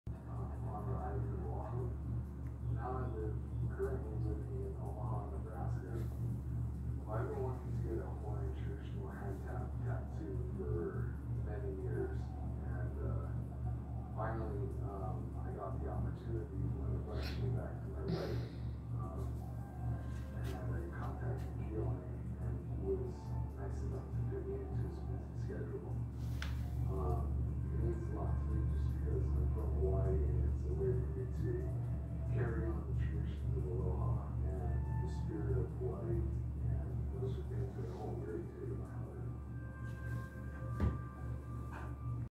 Roadway Inn motel (Metairie) Infestation!
Bathroom Roaches/ Black mold and mildew